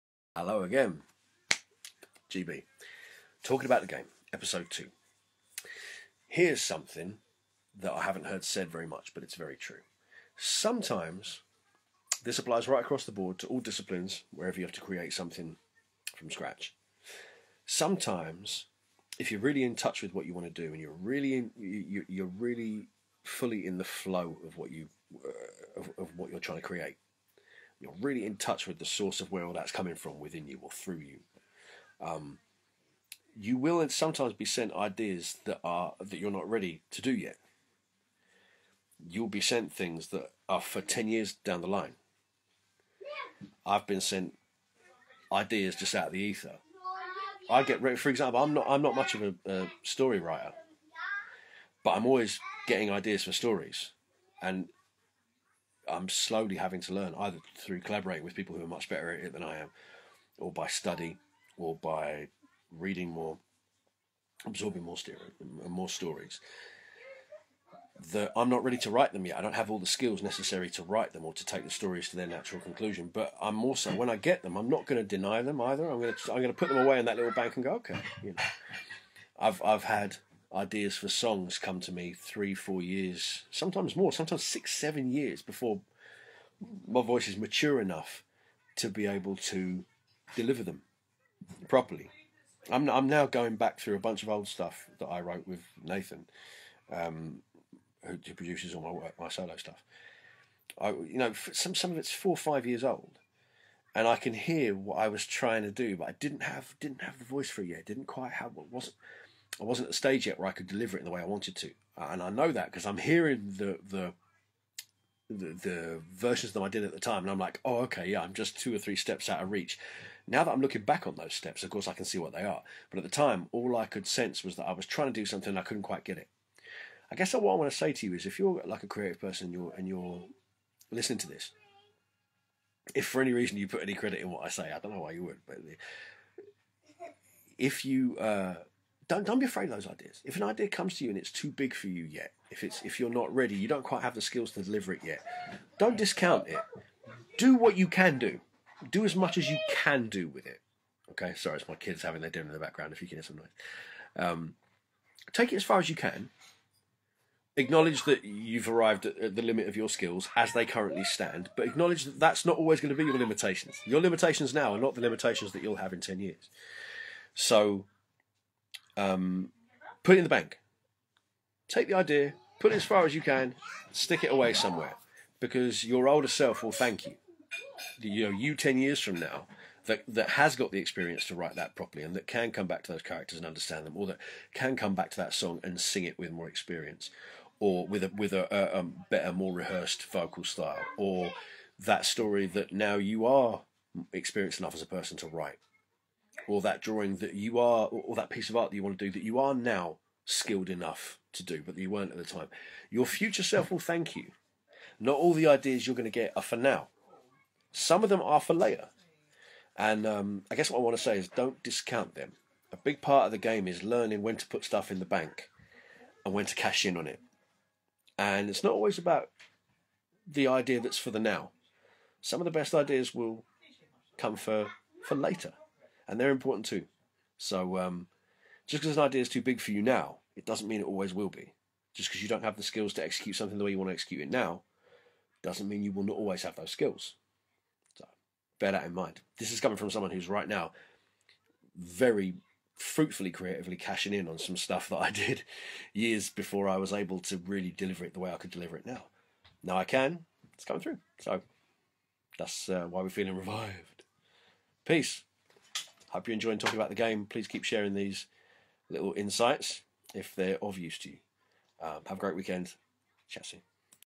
TalkingAboutTheGame Episode 2 - Ideas For The Future
More Sunday thoughts in GB's ongoing TalkingAboutTheGame series.
On this episode we're talking about what to do with ideas for projects that are too big or otherwise out of our reach. Why do we get creative ideas that are beyond our current level of ability? And how should we approach them when we do? Here's some thoughts.